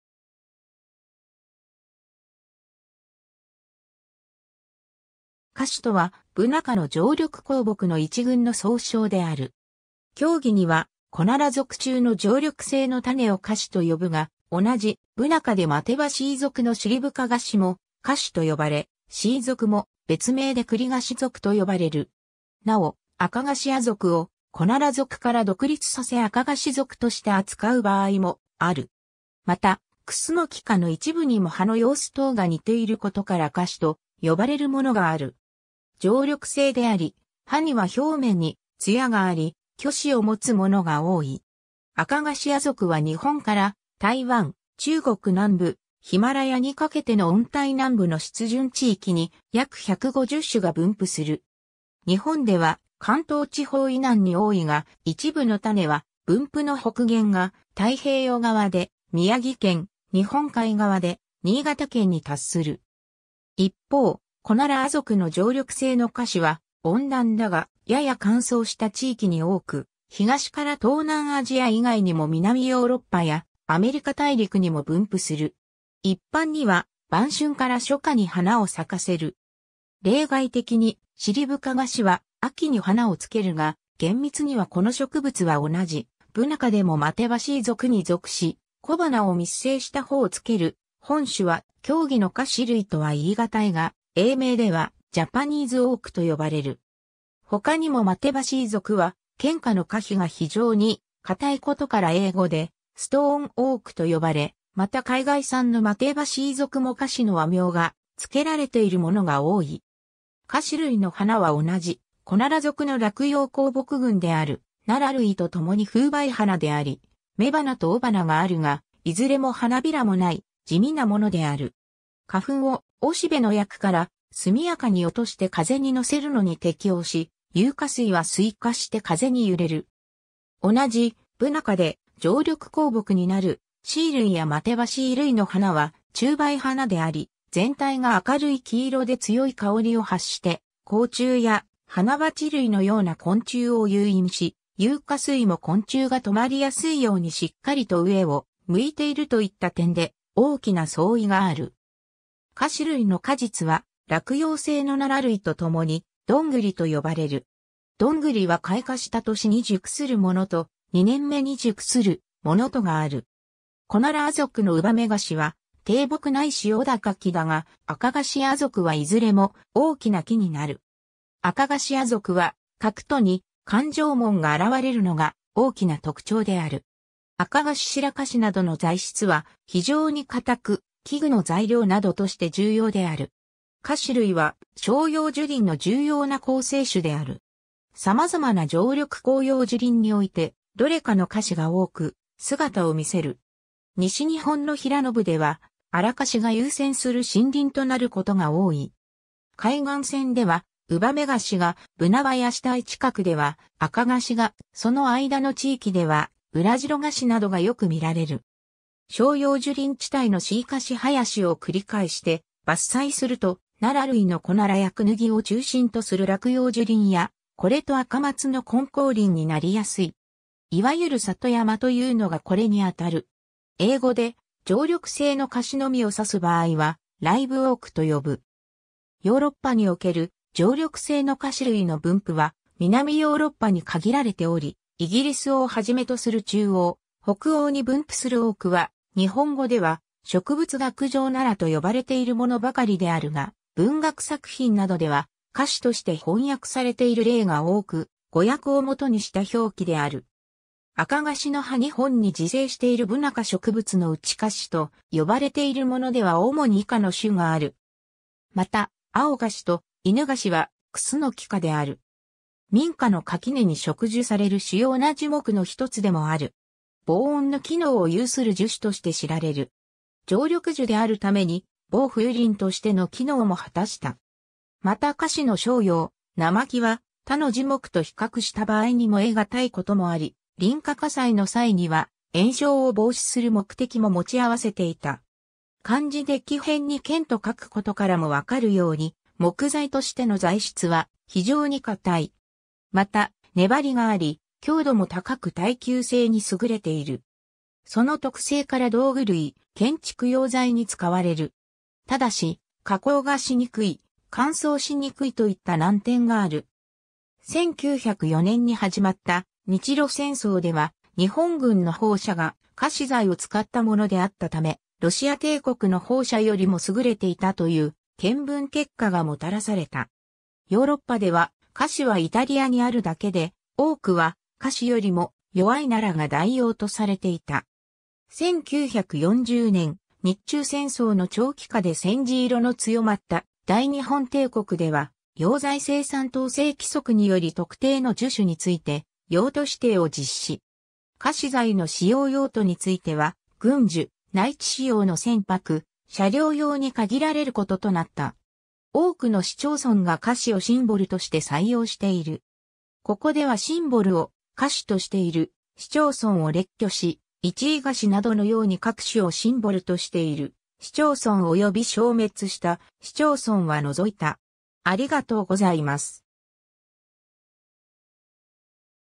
カシ
カシ（樫、橿、櫧）とは、ブナ科の常緑高木の一群の総称である。
狭義にはコナラ属 (Quercus) 中の常緑性の種をカシと呼ぶが、同じブナ科でマテバシイ属のシリブカガシもカシと呼ばれ、シイ属 (Castanopsis) も別名でクリガシ属と呼ばれる。
なお、アカガシ亜属 (subgen. Cyclobalanopsis) をコナラ属から独立させアカガシ属 (Cyclobalanopsis)として扱う場合もある。
またクスノキ科の一部にも葉の様子等が似ていることからカシと呼ばれるものがある。
 常緑性であり、葉には表面につやがあり、鋸歯（葉の縁のギザギザ）を持つものが多い。
アカガシ亜属は日本から台湾・中国南部・ヒマラヤにかけての温帯南部の湿潤地域に約150種が分布する。
日本では関東地方以南に多いが、一部の種は、分布の北限が太平洋側で宮城県、日本海側で新潟県に達する。
一方、コナラ亜属の常緑性のカシは、温暖だがやや乾燥した地域に多く、東から東南アジア以外にも南ヨーロッパやアメリカ大陸にも分布する。
 一般には晩春から初夏に花を咲かせる。
例外的に、シリブカガシは秋に花をつけるが、厳密にはこの植物は同じブナ科でもマテバシイ属に属し、小花を密生した穂をつける、本種は狭義のカシ類とは言いがたいが、英名ではJapanese oakと呼ばれる。
他にもマテバシイ属は堅果（ドングリ）の果皮が非常に硬いことから英語でストーンオーク (Stone oak) と呼ばれ、また海外産のマテバシイ属もカシの和名が付けられているものが多い。
 カシ類の花は同じコナラ属の落葉高木群であるナラ類と共に風媒花であり、雌花と雄花があるが、いずれも花びらもない、地味なものである。
花粉を雄しべの葯から速やかに落として風に乗せるのに適応し、雄花穂は垂下して風に揺れる。
同じブナ科で常緑高木になるシイ類やマテバシイ類の花は虫媒花であり、全体が明るい黄色で強い香りを発して甲虫や花蜂類のような昆虫を誘引し、雄花穂も昆虫が止まりやすいようにしっかりと上を向いているといった点で大きな相違がある。
カシ類の果実は、落葉性のナラ類と共にドングリ（団栗）と呼ばれる。
どんぐりは開花した年に熟するものと、2年目に熟するものとがある。
 コナラ亜属のウバメガシは低木ないし小高木だが、アカガシ亜属はいずれも大きな木になる。
アカガシ亜属は殻斗（いわゆるドングリの皿）に環状紋が現れるのが大きな特徴である。
アカガシ・シラカシなどの材質は非常に堅く、器具の材料などとして重要である。
 カシ類は照葉樹林の重要な構成種である。
様々な常緑広葉樹林において、どれかのカシが多く姿を見せる。
西日本の平野部ではアラカシが優占する森林となることが多い。
海岸線ではウバメガシが、ブナ林帯近くではアカガシが、その間の地域ではウラジロガシなどがよく見られる。
 照葉樹林地帯のシイ・カシ林を繰り返して伐採すると、ナラ類のコナラやクヌギを中心とする落葉樹林や、これとアカマツの混交林になりやすい。
いわゆる里山というのがこれに当たる。
 英語で常緑性のカシのみを指す場合はライブオーク (live oak) と呼ぶ。
ヨーロッパにおける常緑性のカシ類の分布は南ヨーロッパに限られており、イギリスをはじめとする中欧・北欧に分布するoakは、日本語では植物学上ナラ（楢）と呼ばれているものばかりであるが、文学作品などではカシとして翻訳されている例が多く、誤訳を元にした表記である。
 アカガシの葉 日本に自生しているブナ科植物のうちカシと呼ばれているものでは主に以下の種がある。
 また、アオガシとイヌガシはクスノキ科である。
 民家の垣根に植樹される主要な樹木の一つでもある。
防音の機能を有する樹種（防音樹）として知られる。
 常緑樹であるために防風林としての機能も果たした。
またカシの生葉・生木は他の樹木と比較した場合に燃え難いこともあり、隣家火災の際には延焼を防止する目的も持ち合わせていた。
 漢字で木偏に堅と書くことからも判るように、木材としての材質は非常に堅い。
また粘りがあり強度も高く耐久性に優れている。
その特性から道具類、建築用材に使われる。
ただし、加工がしにくい、乾燥しにくいといった難点がある。
 1904年に始まった日露戦争では、日本軍の砲車がカシ材を使ったものであったため、ロシア帝国の砲車（ヤシャブシで代用していた）よりも優れていたという見分結果がもたらされた。
ヨーロッパでは、カシはイタリアにあるだけで、多くはカシよりも弱...